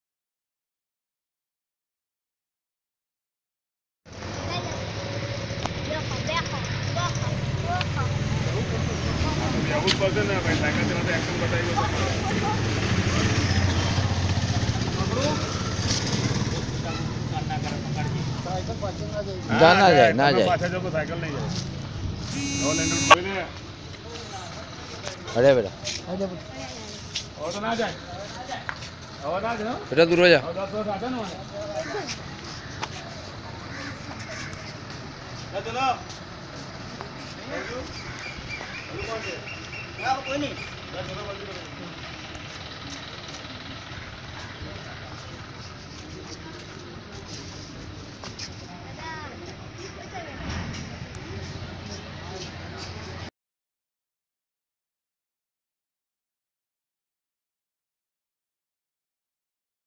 An attack of hysteria took this man's control of his legs...
Challenge:
Miyasab is a 60 year old widower who lives with his daughter’s family. Due to an attack of hysteria four years ago, he lost balance of both legs and is now bedridden.

Solution:
Our supporter Smile Plus Dental, Edison provided a tricycle that will not only help Miyasab travel to the hospital, but also provide him with the means to move around along with the ability to think about ways to make good use of his time.

Donor:
Smile Plus Dental, Edison, New Jersey

Location:
Khatrej Darvaja Mahemdavad, Gujarat, India